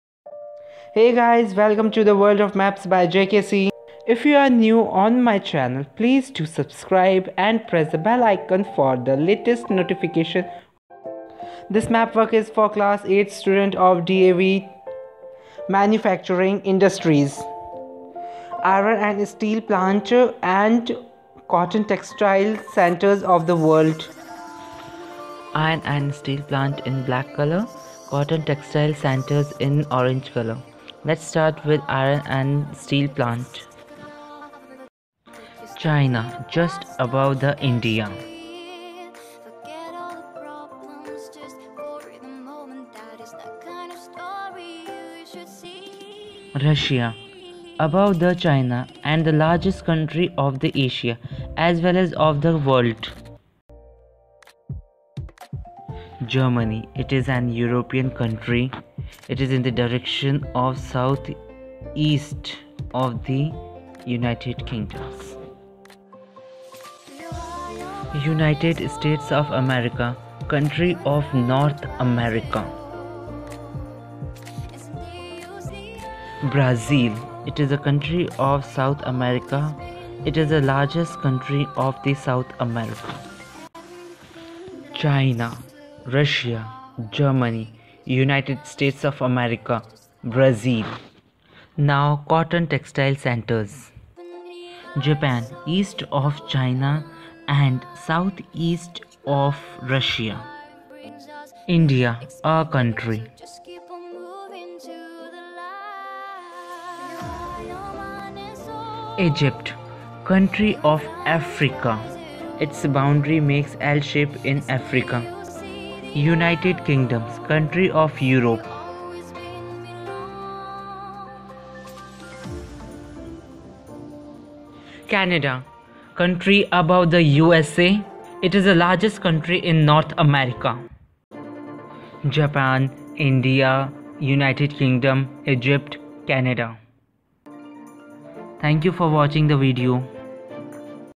Class 8 Geography Manufacturing Industries
DAV BOARD CLASS 8
SOCIAL STUDIES
GEOGRAPHY (Map Work on Political Map of the World)
CHAPTER 6 : MANUFACTURING INDUSTRIES
IRON & STEEL PLANTS : CHINA, RUSSIA, GERMANY, USA, BRAZIL
COTTON TEXTILE CENTRES : JAPAN, INDIA, EGYPT, UK, CANADA